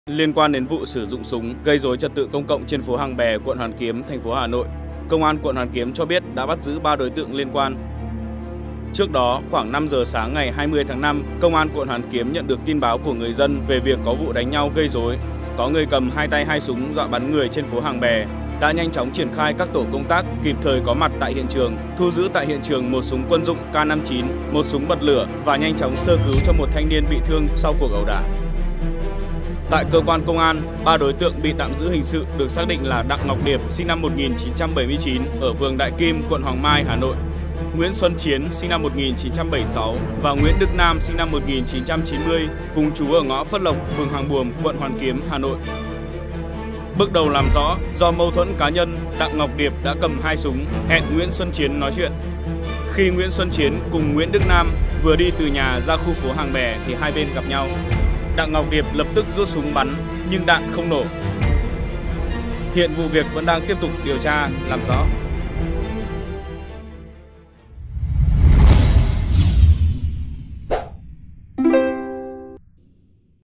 Danh tính 3 đối tượng cầm s.ú.n.g gây rối ở phố Hàng Bè | VTC14
VTC14 | Liên quan đến vụ sử dụng s.ú.n.g gây rối trật tự công cộng trên phố Hàng Bè, quận Hoàn Kiếm TP Hà Nội. Công an quận Hoàn Kiếm cho biết đã bắt giữ 3 đối tượng liên quan.